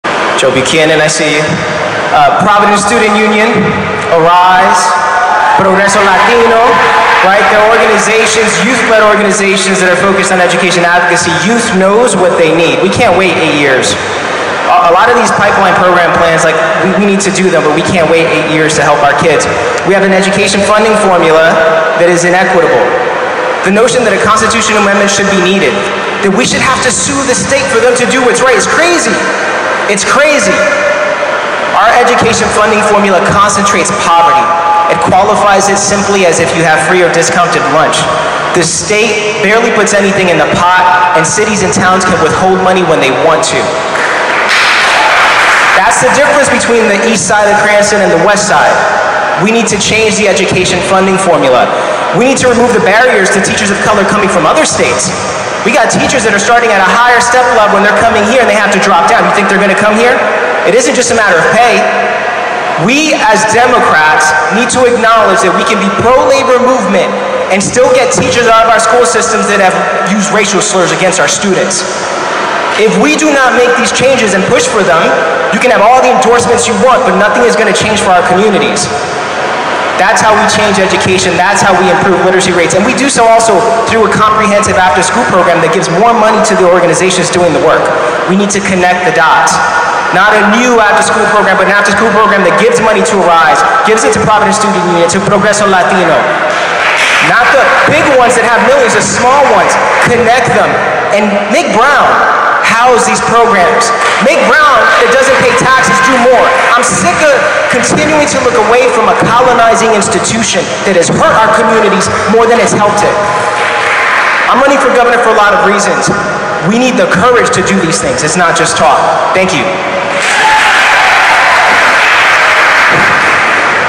John Hope Forum: Bold Calls to Transform RI Education Now!
Candidates and advocates discuss critical issues in Rhode Island’s education system during the John Hope Gubernatorial Forum. Joe Buchanan, representing the Providence Student Union and ARISE, highlights the urgent need for reform in the state’s education funding formula, which he argues perpetuates inequality. He points out the disparities between areas like the east and west sides of Cranston and calls for removing barriers for teachers of color coming from other states. Buchanan also emphasizes the importance of supporting grassroots organizations like ARISE and the Providence Student Union over larger, well-funded programs. The forum underscores the need for comprehensive after-school programs and accountability within the school system.